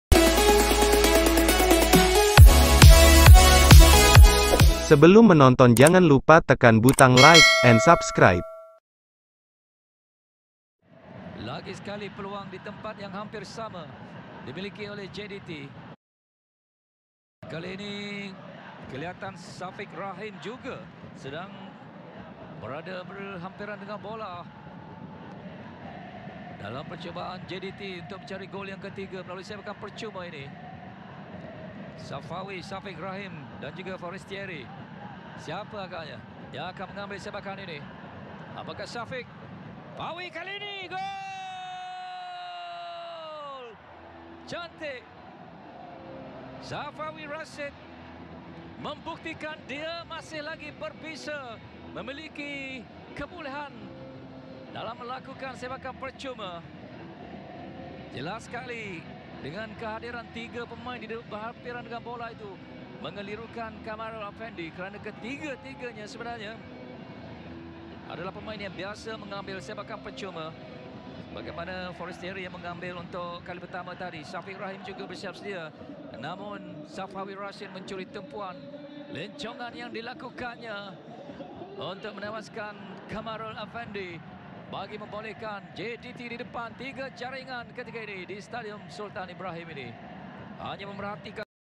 ''SAFAWI IS BACK'' FREE KICK CANTIK DARI SAFAWI RASID MENENTANG BRM FC | PIALA FA MALAYSIA...
GOL 3 DI JARINGKAN OLEH SAFAWI RASID MELALUI FREE KICK YANG CANTIK.